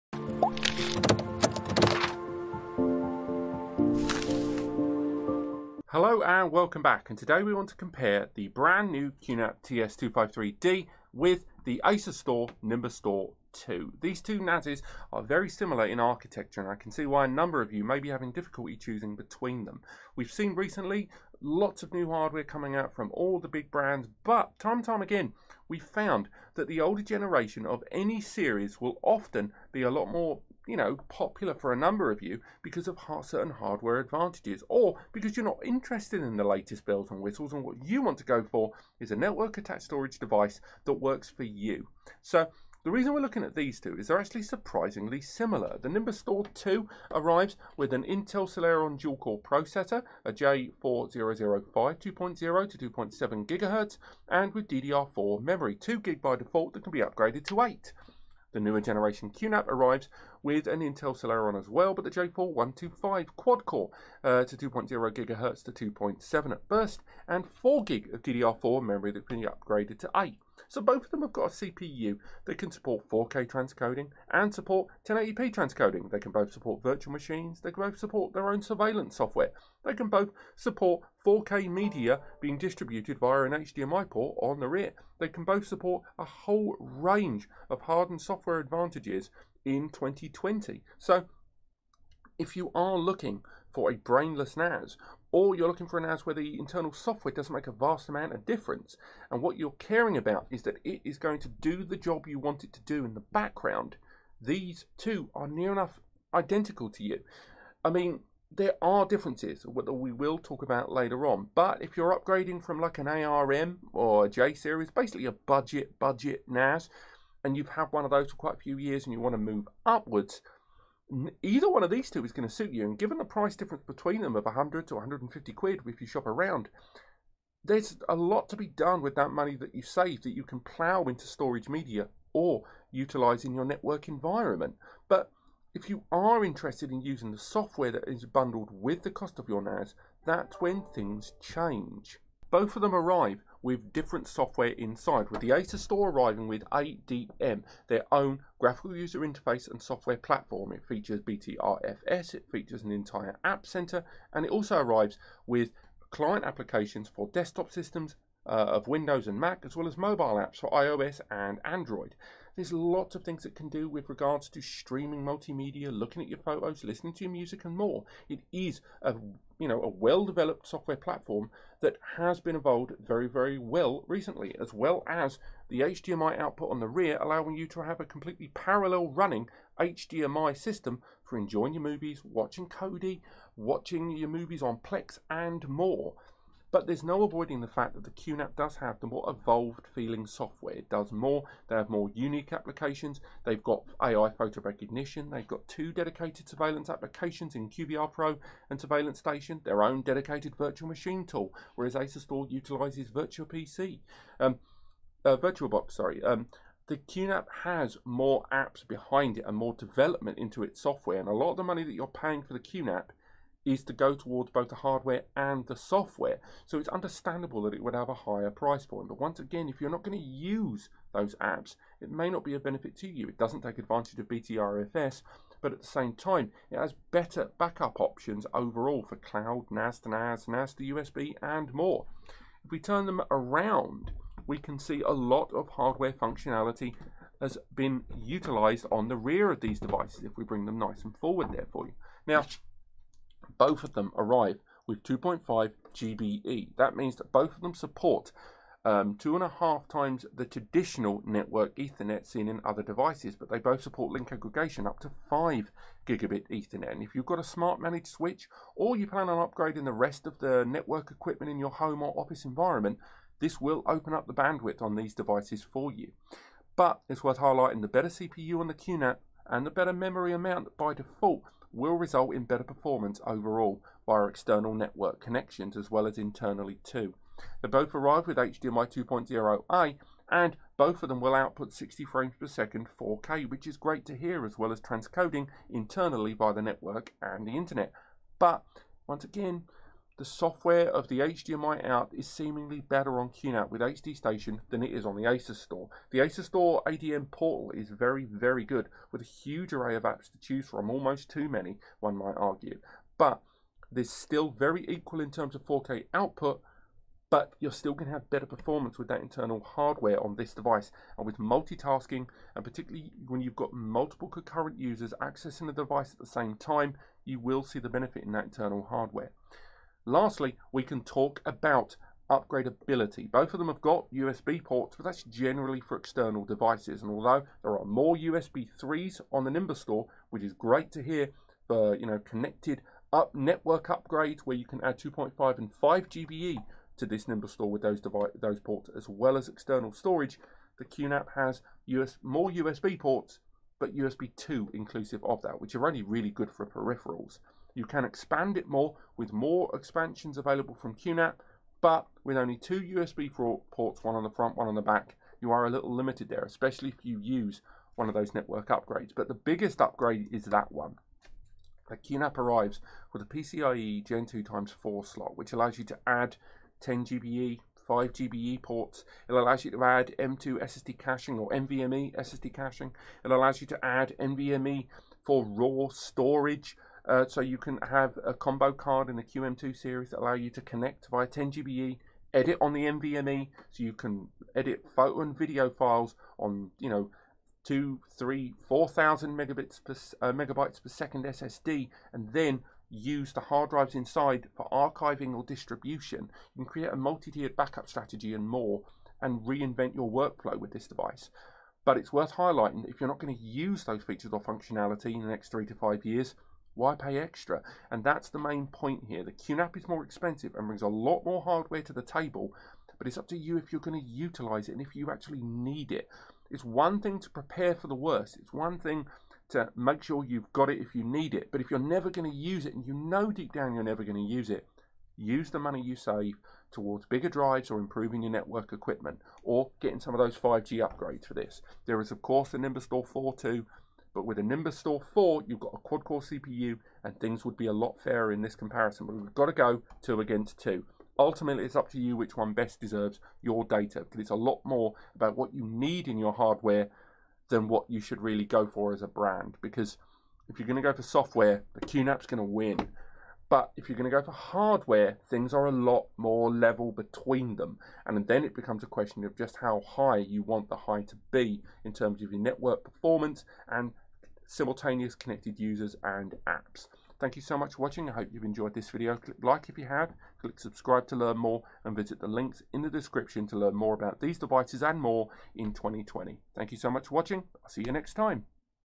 QNAP TS-253D vs Nimbustor 2 NAS Drive Comparison
Buying your first Network Attached Storage (NAS) device can be a remarkably daunting task! Even for the most technologically knowledgable, a NAS is a very different kind of device than most of the hardware in your environment. At its heart, it is just a computer (like most things are when you break them down to the basics) that is built for a specific task. However, there are numerous elements of a NAS (such as it always being ON, the constant remote accessibility and focus on storage primarily) that set it apart from most other devices, and this si where a lot of people get a bit intimidated by it. There is always the option of using cloud services, such as DropBox, Google Drive, OneDrive and more, but these are often paid monthly subscription services, involve your data being shared across other services and (most worrying of all) leaves your precious data in a position to be used for analytics without your knowledge OR open to hacking because of the sheer size of the target. Add to this the fact that in most cases, a severed connection to the internet means a severed connection to your data, and the appeal of moving away from 3rd party cloud and on to a NAS is pretty clear. The industry of NAS has been in constant growth for over 30 years, with big players like HP, Dell and IBM steadily losing the home and SMB market to the likes of Synology, QNAP and more. The brand diversity in the field of NAS servers has continued to grow right up until 2021, with the benefit that the way you can interact with your data, what NAS devices can do and the price point of this technology being exceedingly affordable (with fully featured solutions WITH storage media arriving as low as £200). So, today I want to talk about what NAS is, what a NAS can do, and which NAS Brand you should consider buying in 2021.

Still not enough? Updated regularly it provide you with hints and tips on how to make the most of your hardware